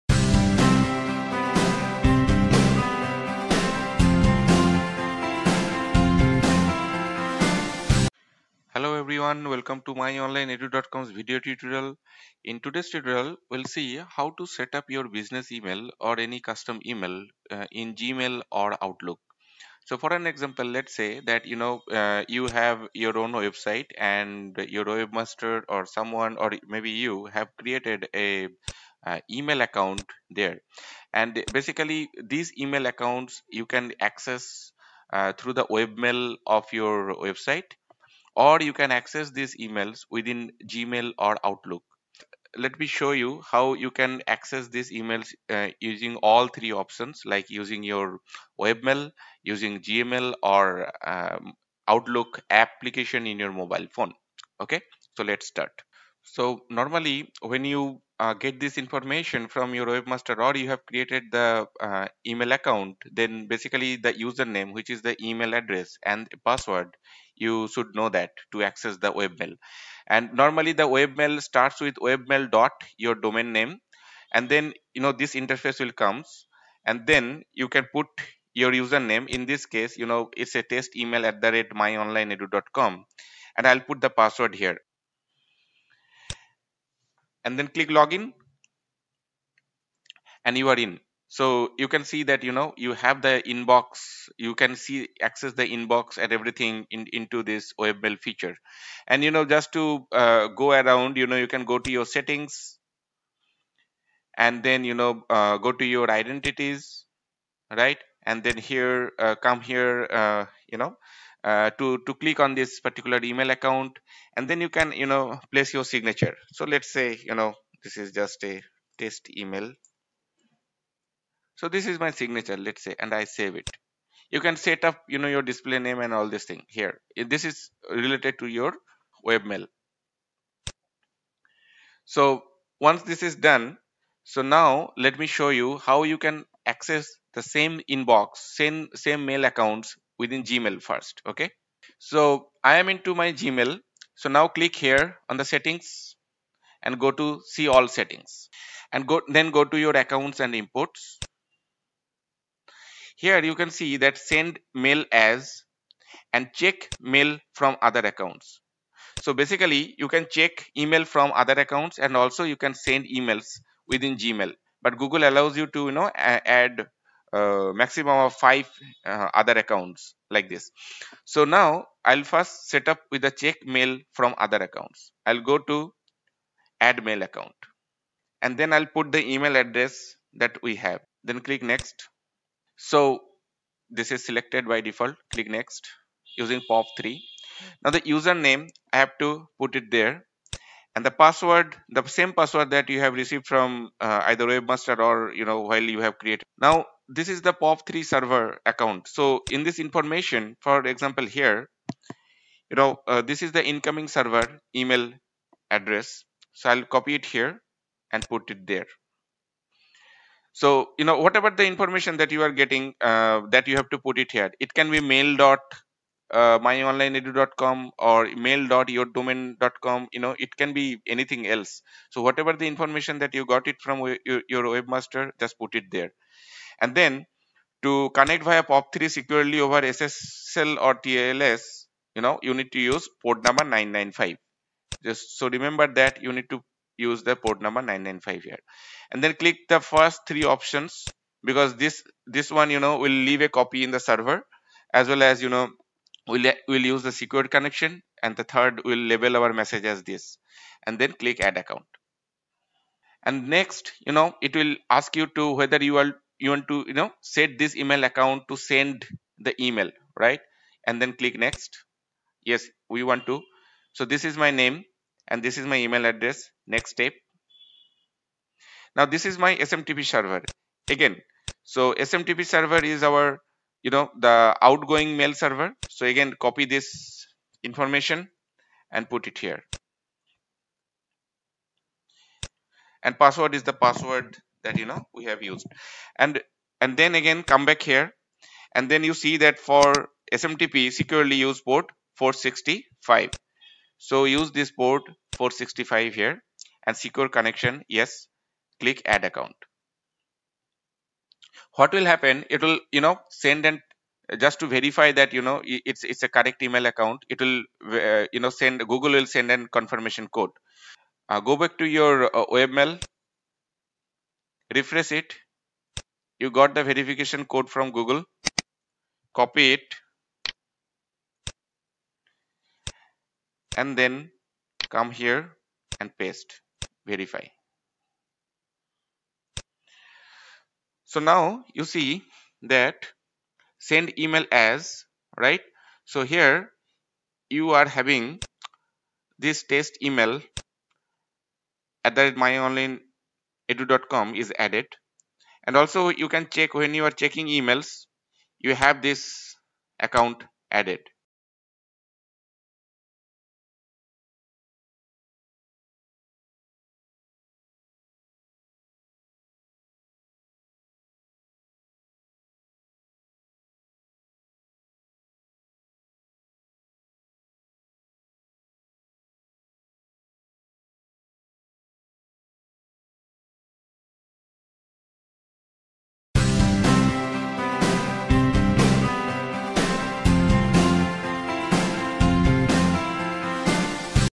Setup Custom Email or External Email to Gmail or Outlook App
This video will guide you to setup your custom email in Gmail or Outlook app.

You need to use the information for incoming mail server (POP3 port 995) and outgoing SMTP server (port 465).